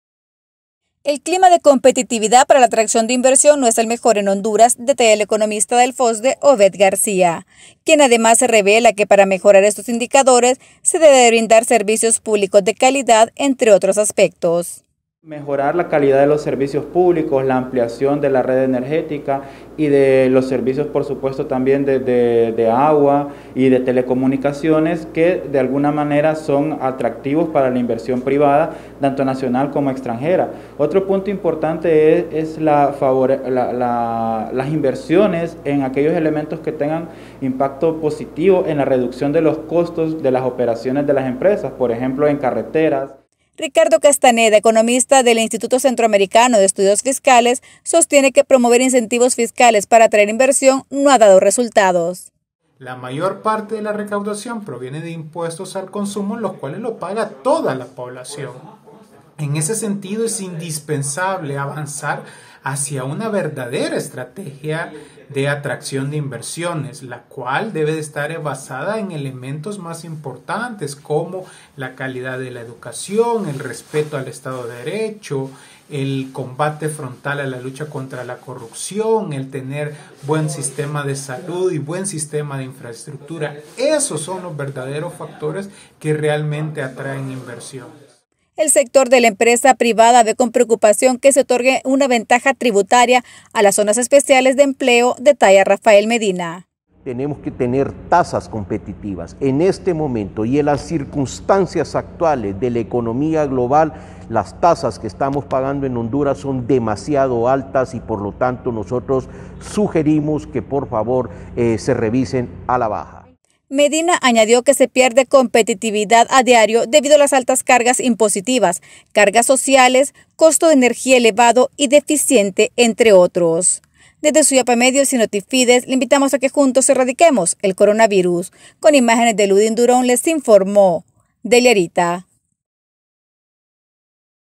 Índices de competitividad
Honduras es el país de la región centroamericana con los índices más bajos de competitividad revelan economistas y empresarios.